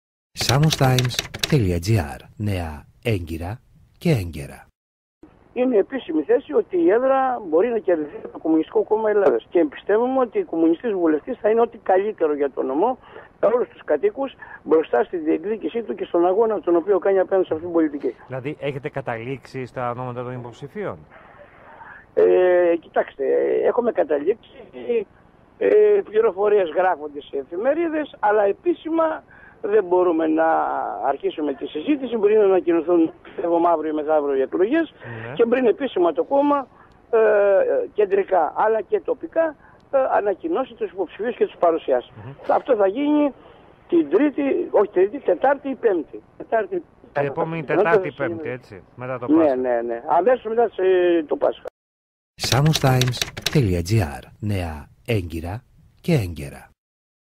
ΝΙΚΟΣ ΣΟΡΟΚΟΣ ΚΚΕ
Ιδιαίτερα αισιόδοξα ότι μπορεί να κερδηθεί η έδρα της Σάμου εμφανίζονται στελέχη του Κ.Κ. στο νησί μας.
Μιλώντας στο Σάμος FM 97, 3  ο Γραμματέας της Νομαρχιακής Επιτροπής του κόμματος τόνισε ότι υπάρχει αυτή η δυναμική ενώ σε ότι αφορά στα ονόματα που θα απαρτίσουν το ψηφοδέλτιο θα ανακοινωθούν και θα παρουσιαστούν σε ανοιχτή εκδήλωση την επόμενη εβδομάδα.